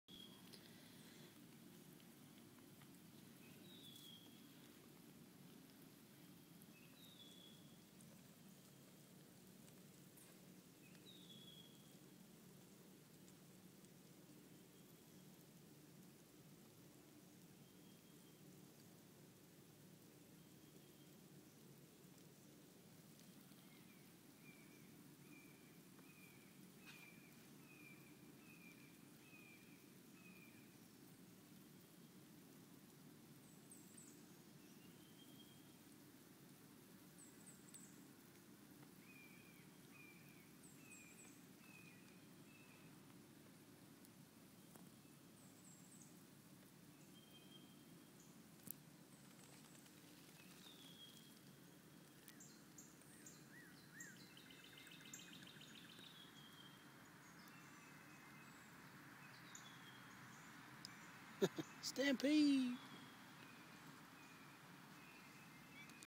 Ant march
Came across this very efficient move. I don't know why but they packed up a headed for the wood line. Thought it would be neat to share.